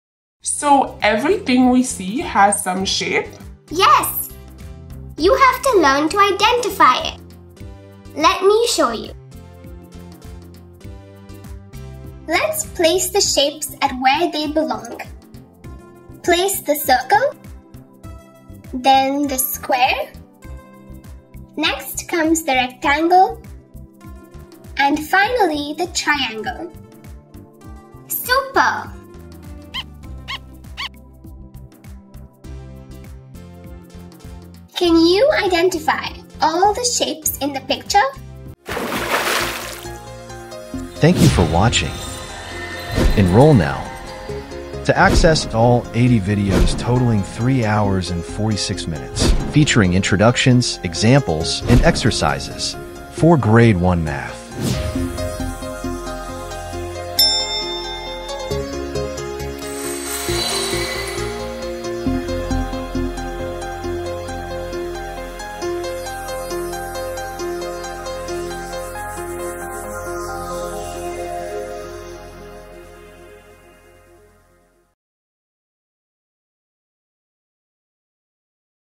G1 - Module 6 - Exercise 1 - Identify Shapes | Appu Series | Grade 1 Math Learning
Identify Shapes: Explore the exciting world of shapes as you learn what each one is called! Have fun while you exercise your math and observation skills!